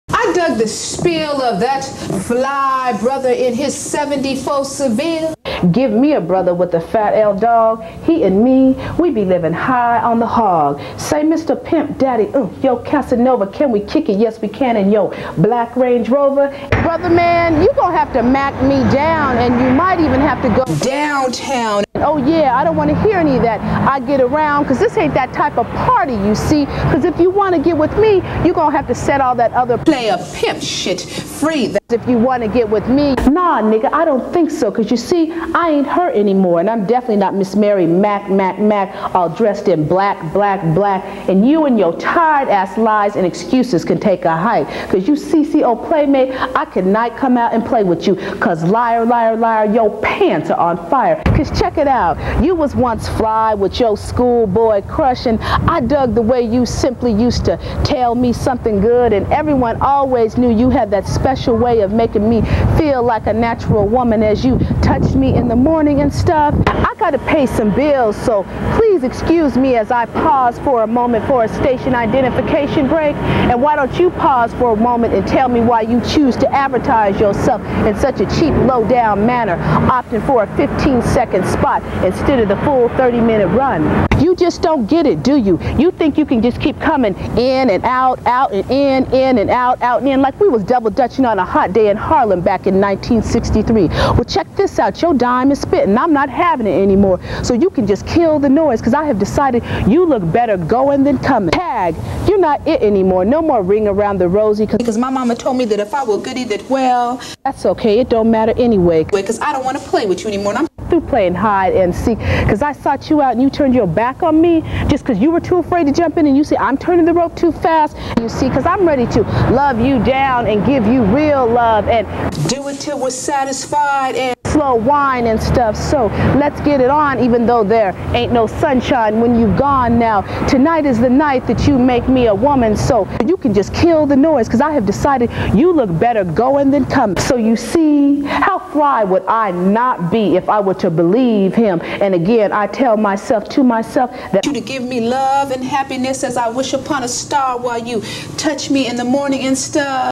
Roni talkin' shyt... 1995..Roni's Bakstreet Poetri
This is clip of Roni in various locations around Hollywood...including the basement of my second record store...as alwayz she was talkin' shyt...someone body broke into my truck at the time and sista thought I was lying! Anyway we both were lucky that I was able to have some footage on her...